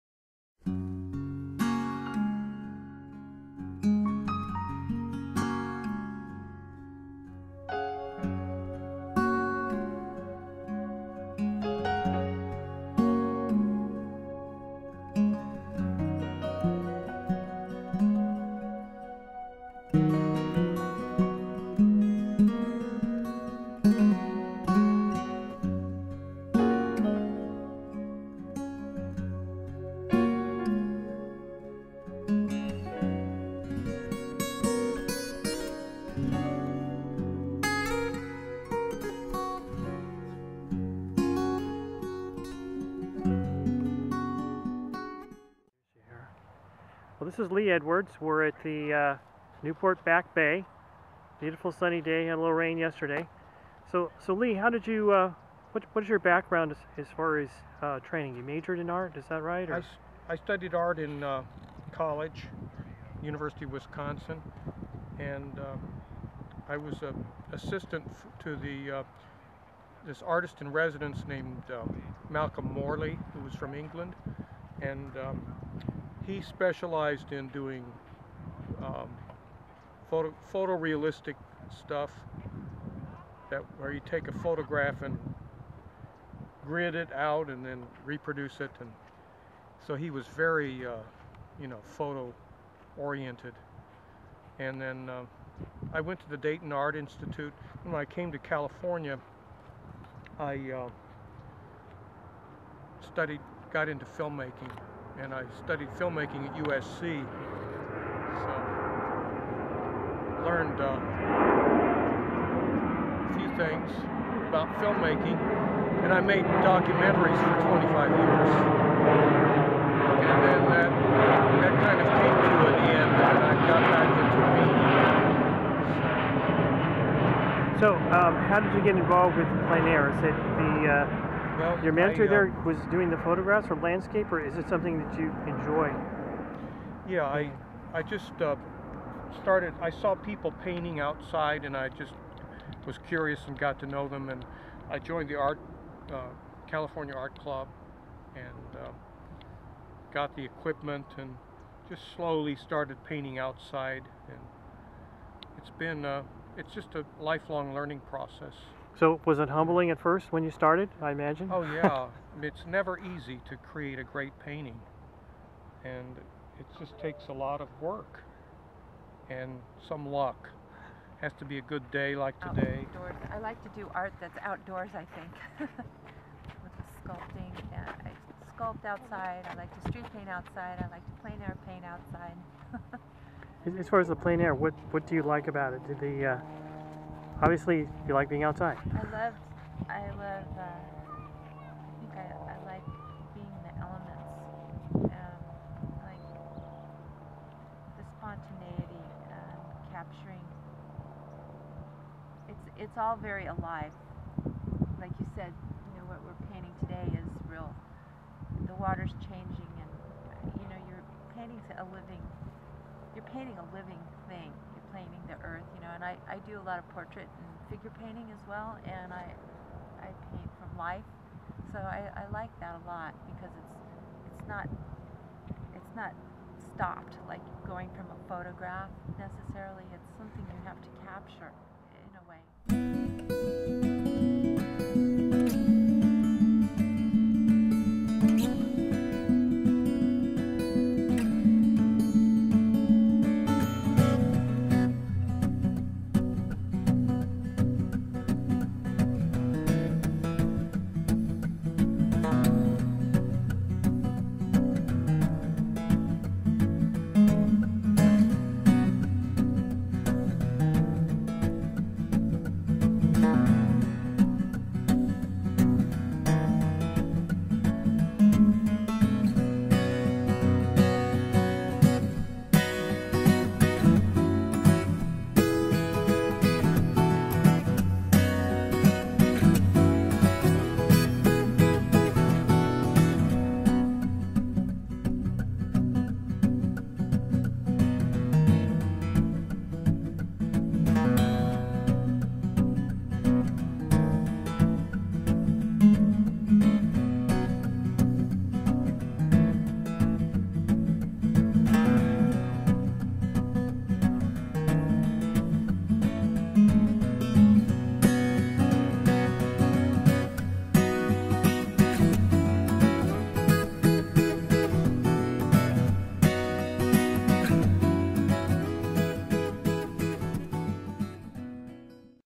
DH Team sponsored Newport Beach Back Bay Plein Air Paint Out 12/15/12
DH Team llc sponsored Newport Beach Back Bay Plein Air Paint Out Saturday 12/15/12. It was a wonderful sunny day with huge tide changes with very talented Artists from the L.A. area. The wild & plant life are amazing for such a small area. All photos were shot on location that day.  If you are interested in DH Team events, Art Gallery showings, or Plein Air Art please call Mark Davey at .